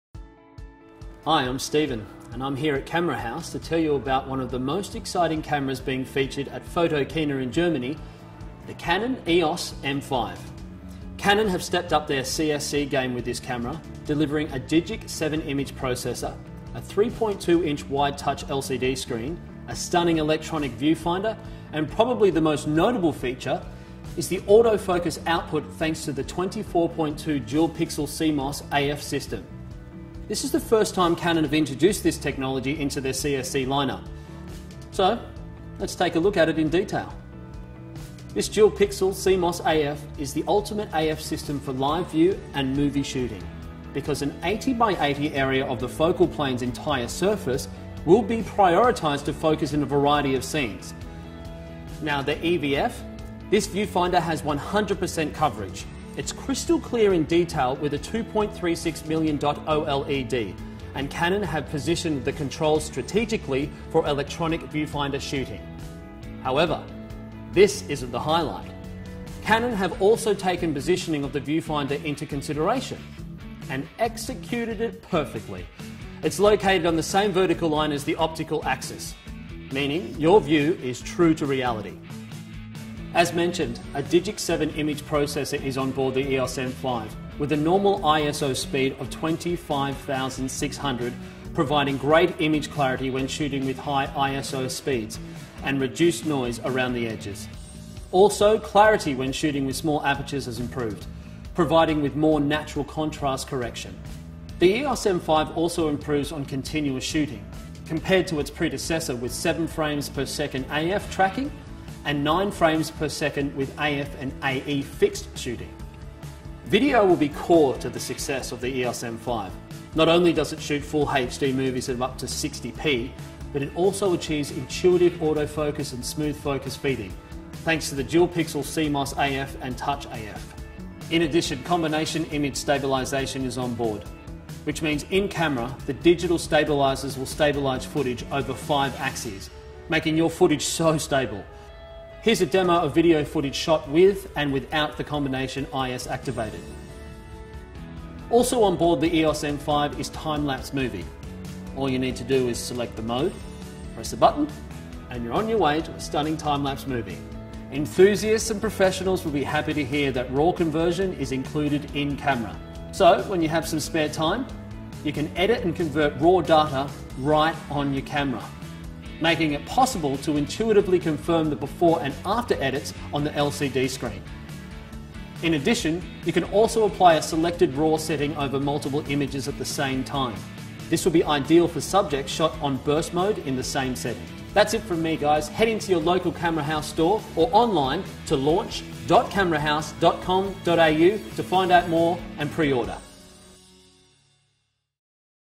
Canon EOS M5
Canon has just upped the Compact System Camera Game with the announcement of the Canon EOS M5.
- DiG!C 7 Image Processor - 3.2” Wide Touch LCD Screen - Electronic Viewfinder with 100% coverage - 24.2 Dual Pixel CMOS AF system